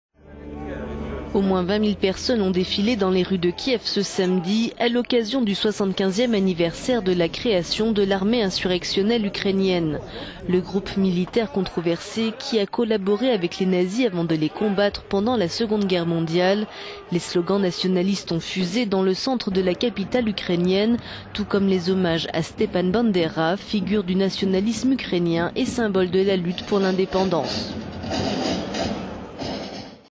Mobilisation nationaliste en Ukraine
Au moins 20 000 personnes ont défilé dans les rues de Kiev, ce samedi, à l'occasion du 75e anniversaire de la création de l'Armée insurrectionnelle ukrainienne, le groupe militaire controversé qui a collaboré avec les nazis avant de les combattre pendant la Seconde Guerre mondiale.